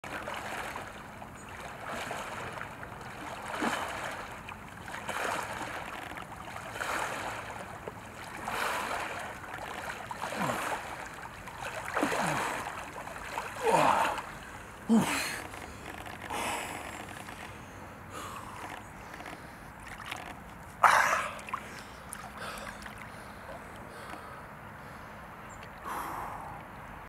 Bei Wind und Wetter: Kai Diekmann badet in der Havel
Herbst 2020 in Potsdam: Es ist grau, die Lufttemperaturen sind knapp zweistellig und das Wasser der Havel ist ebenso kalt. Für den ehemaligen Bild-Chefredakteur Kai Diekmann kein Problem, wie man sieht